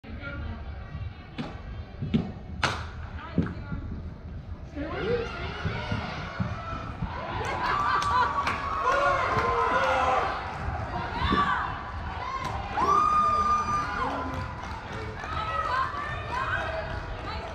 Stealing all the bases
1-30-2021, USFA Winter Classic, Noblesville, IN, Bracket game 1
Northern Indiana Elite vs Indiana Beasts 07